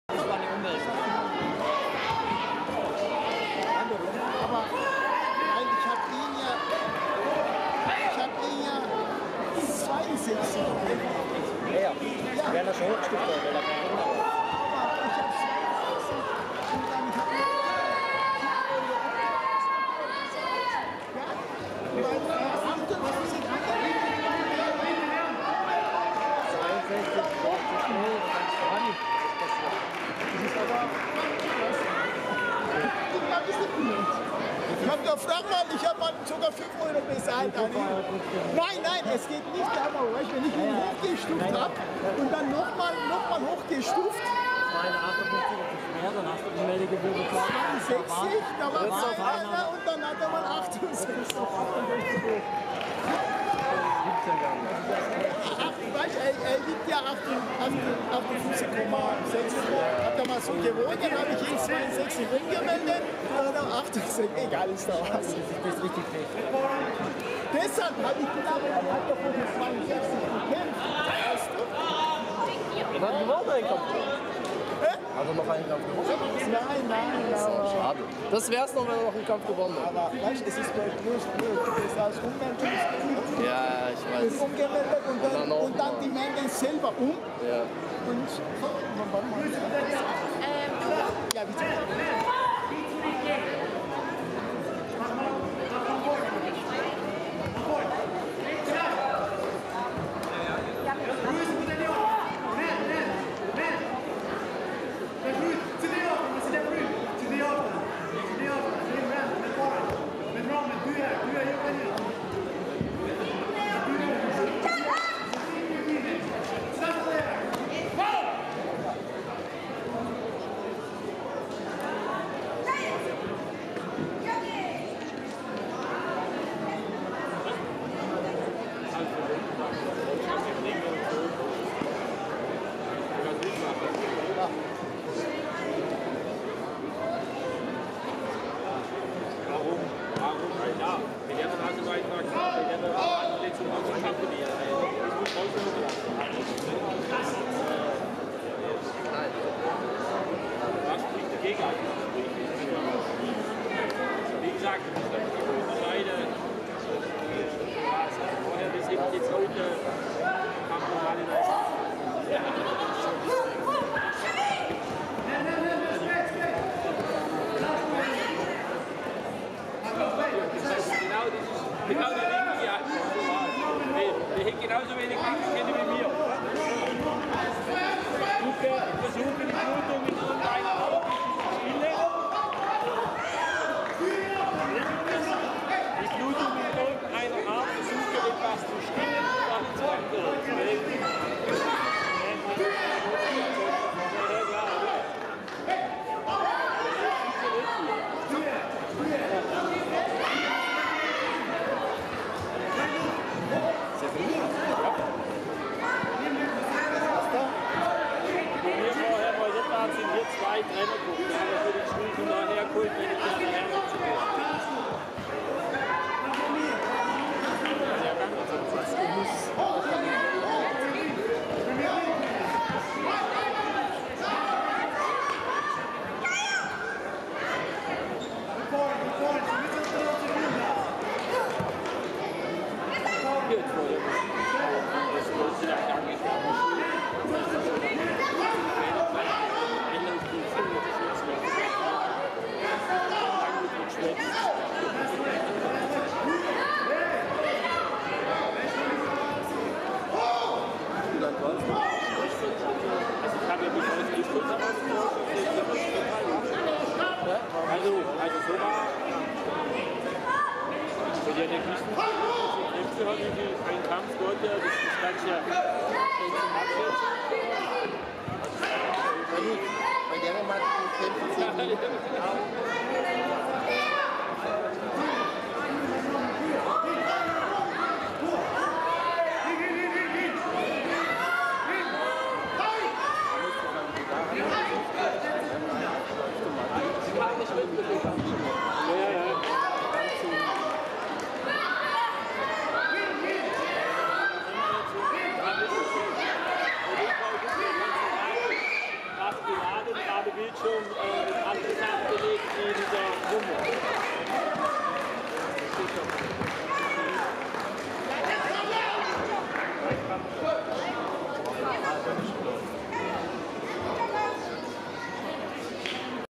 16 TUTTLINGER TAEKWONDO TURNIER 2019 248 Leon Demhasaj SUI vs Niklas Mellenthin GER
16 INTERNATIONALES TUTTLINGER TAEKWONDO TURNIER 2019